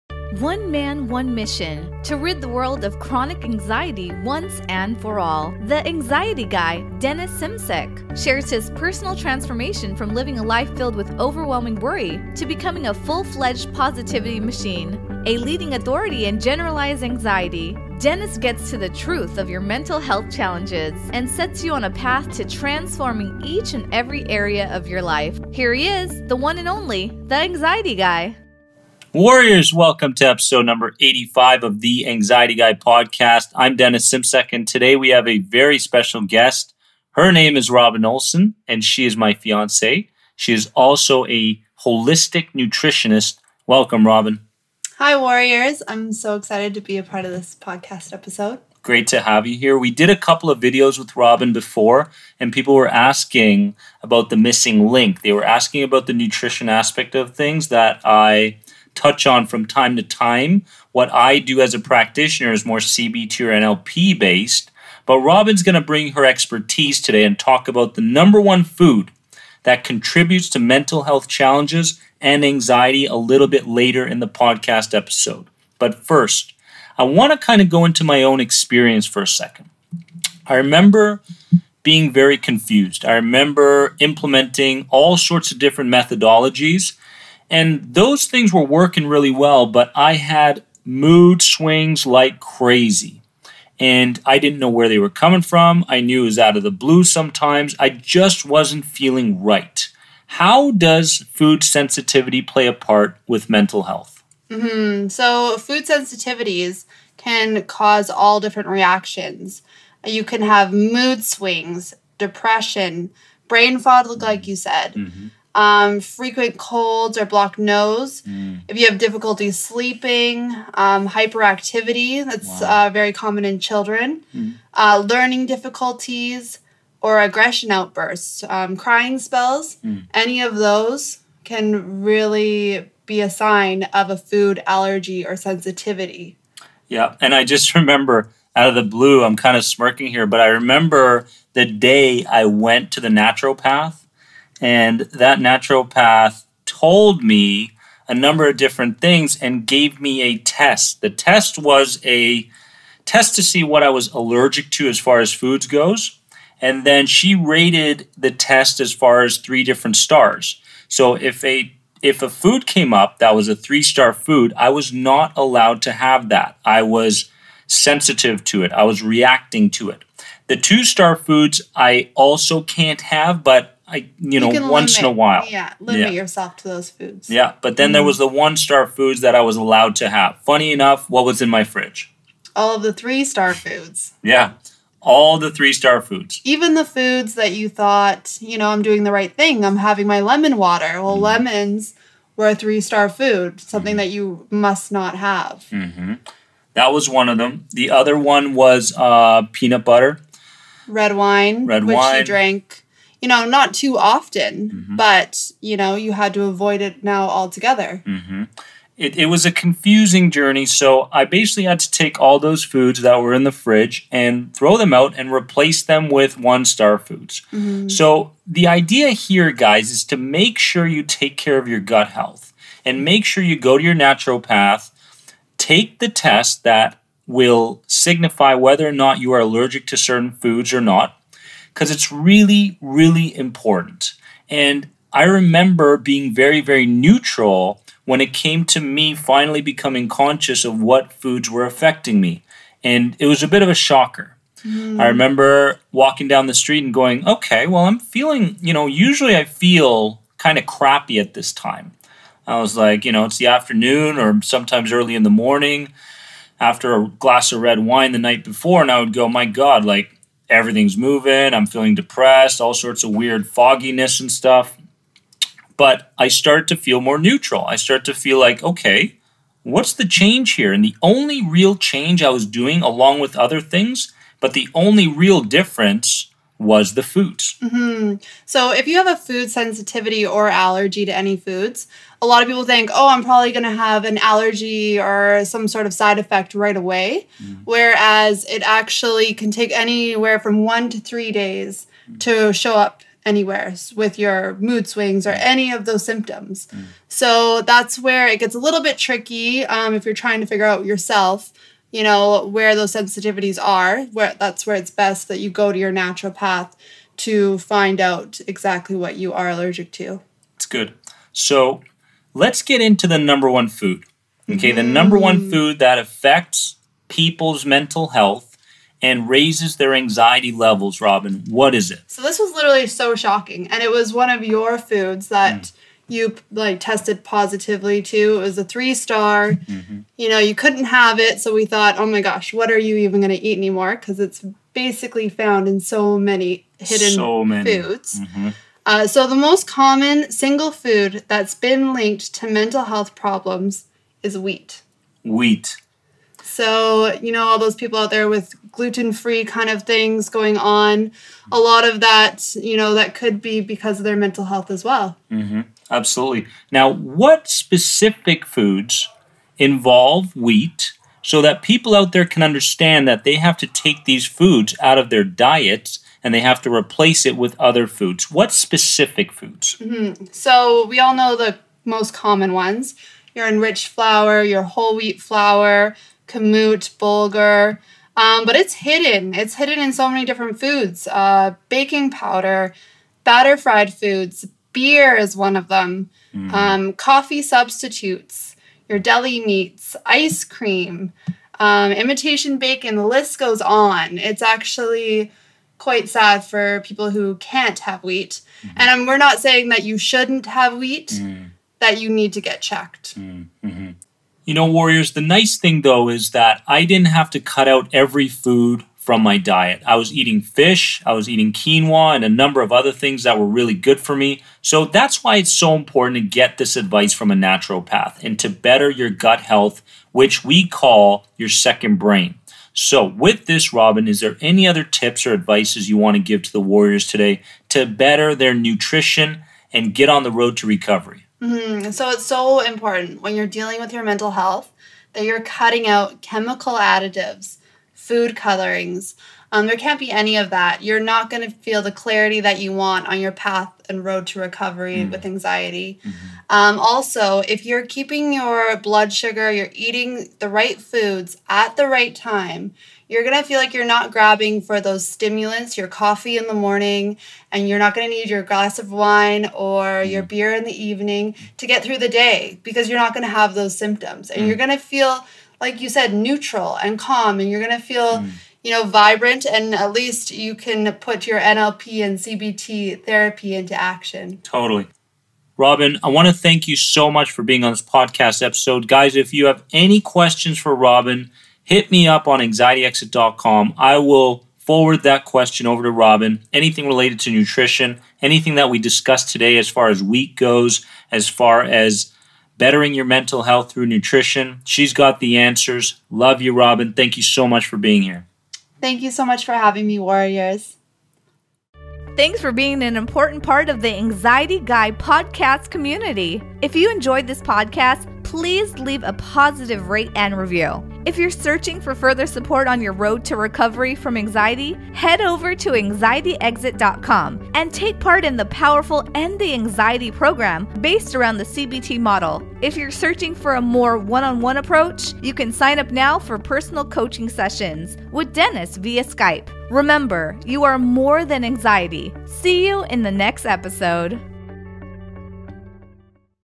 The #1 Food That's Contributing To Your Anxiety (SHOCKING) / Podcast #85
to begin your natural recovery from anxiety through the #1 CBT program online.

Description: In episode #85 of The Anxiety Guy Podcast me and my special guest my fiancee Robyn who specializes in holistic nutrition get together to give you the #1 food that is contributing to your anxiety the most.

Imagine eating what you think is good healthy foods, only to come up with the conclusion that it may be the cause for your anxiety levels. This was me as I mention deeply in this podcast episode the moments I visited my local naturopath.

Little did I realize at the time how certain food allergies caused my anxiety levels to spike, and so the journey began to find proper replacements. In episode #85 of the anxiety guy podcast Robyn weighs in on her recommendations as far as what to eat, and what sort of foods to stay away from (if in fact you may have a food sensitivity causing your anxiety).

We also weigh in on the importance of your gut health, commonly known as your 2nd brain. So get empowered, get your thinking cap on, and get ready to join us for this powerful anxiety guy podcast episode on foods that contribute to your anxiety levels, and mental health challenges.

Please Share With Anyone Who Is Currently Suffering From Generalized Anxiety Or Health Anxiety!